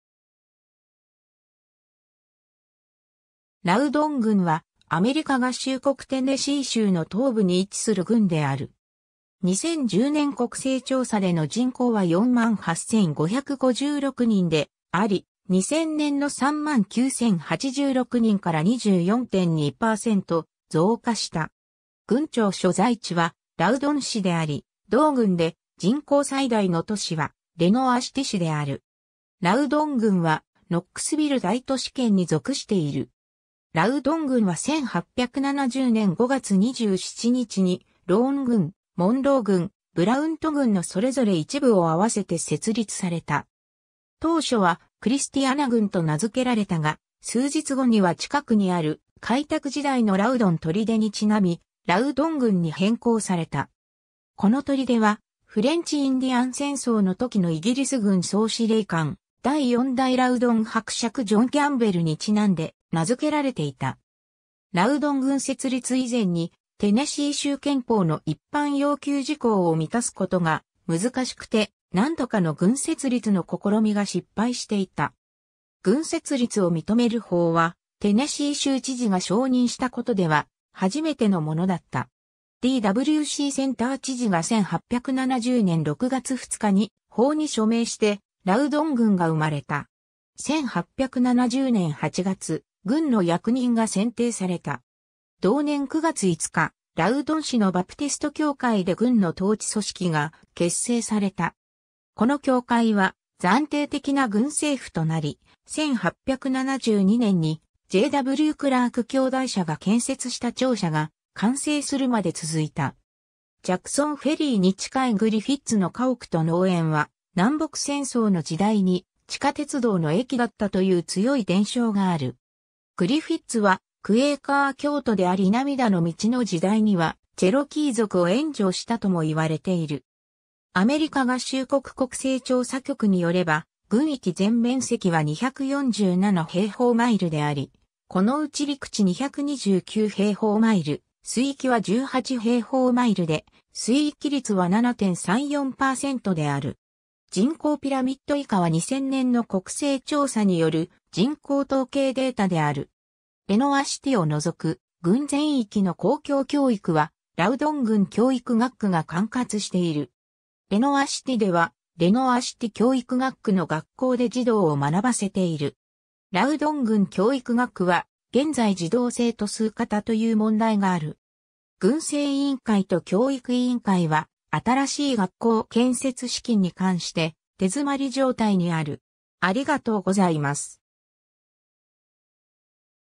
ラウドン郡 (テネシー州)
ラウドン郡（英: Loudon County）は、アメリカ合衆国テネシー州の東部に位置する郡である。
2010年国勢調査での人口は48,556人であり、2000年の39,086人から24.2%増加した。
郡庁所在地はラウドン市（人口5,381人）であり、同郡で人口最大の都市はレノアシティ市（人口8,642人）である。
 ラウドン郡はノックスビル大都市圏に属している。
 ラウドン郡は1870年5月27日に、ローン郡、モンロー郡、ブラウント郡のそれぞれ一部を合わせて設立された。
当初はクリスティアナ郡と名付けられたが、数日後には近くにある開拓時代のラウドン砦に因み、ラウドン郡に変更された。
この砦は、フレンチ・インディアン戦争の時のイギリス軍総司令官第4代ラウドン伯爵ジョン・キャンベルに因んで名付けられていた。
ラウドン郡設立以前に、テネシー州憲法の一般要求事項を満たすことが難しくて、何度かの郡設立の試みが失敗していた。
郡設立を認める法はテネシー州知事が承認したことでは初めてのものだった。
D・W・C・センター知事が1870年6月2日に法に署名して、ラウドン郡が生まれた。
 1870年8月、郡の役人が選定された。
同年9月5日、ラウドン市のバプテスト教会で郡の統治組織が結成された。
この教会は暫定的な郡政府となり、1872年にJ・W・クラーク兄弟社が建設した庁舎が完成するまで続いた。
 ジャクソンフェリーに近いグリフィッツの家屋と農園は、南北戦争の時代に地下鉄道の駅だったという強い伝承がある。
グリフィッツはクエーカー教徒であり涙の道の時代にはチェロキー族を援助したとも言われている。
 アメリカ合衆国国勢調査局に拠れば、郡域全面積は247平方マイル (640 km2)であり、このうち陸地229平方マイル (594 km2)、水域は18平方マイル (47 km2)で水域率は7.34%である。
 レノアシティを除く郡全域の公共教育はラウドン郡教育学区が管轄している。
 レノアシティではレノアシティ教育学区の学校で児童を学ばせている。
ラウドン郡教育学区は現在児童生徒数過多という問題がある。
郡政委員会と教育委員会は新しい学校建設資金に関して手詰まり状態にある。